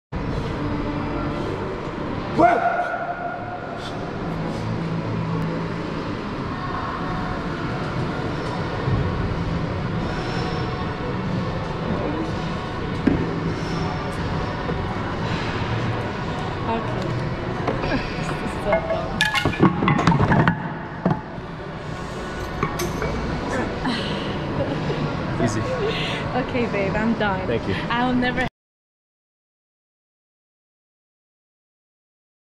Dips PR 1 x 95kg (209 lbs)
Weighted dips personal record 1 x 95kg (209 lbs) RPE 8.5 at 79kg bodyweight.